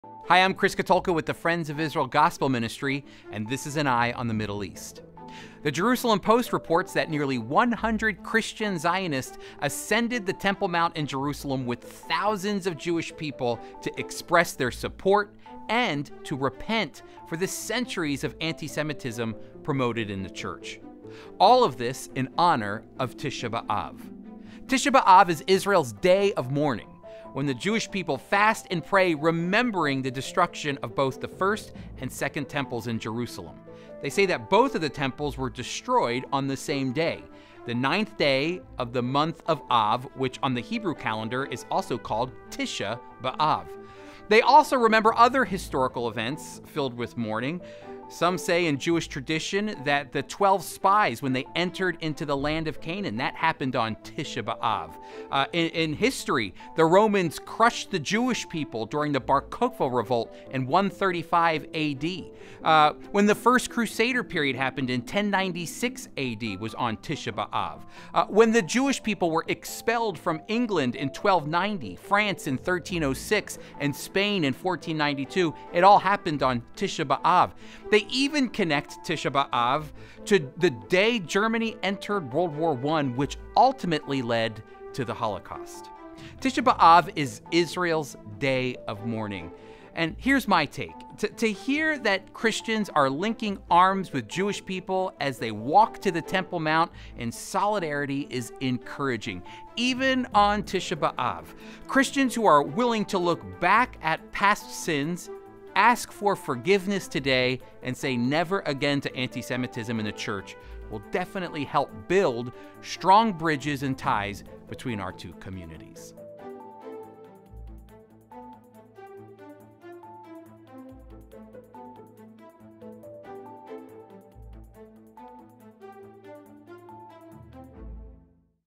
Eye on the Middle East: Christians Show Solidarity with Jewish People on Tisha B’Av
Hundreds of Christians travel to Jerusalem during Tisha B’Av to show support to Israel and the Jewish people.